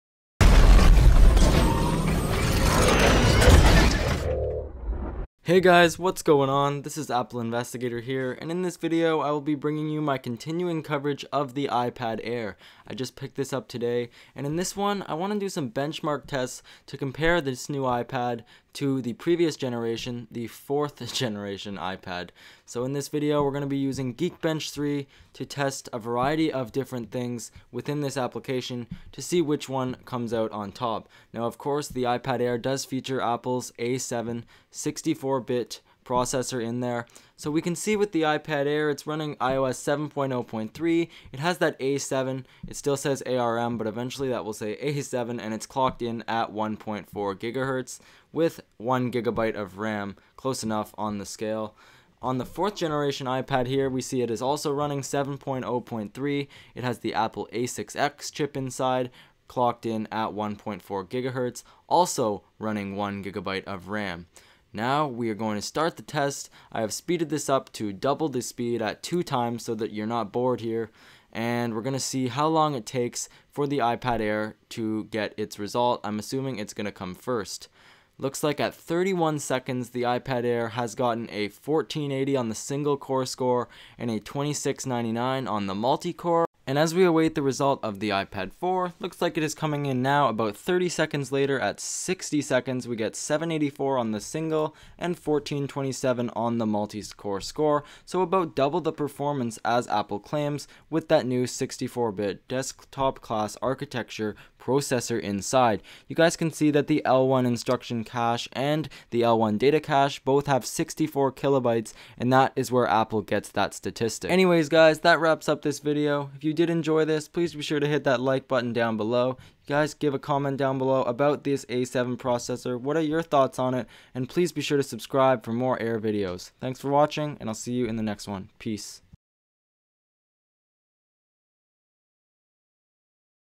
iPad Air VS. iPad 4 (A7 vs A6X) - Any Difference?
In this video, I show you guys a geek bench comparison between the iPad 4th Generation and the brand new iPad Air! Thanks!
Fullscreen: Fullscreen.net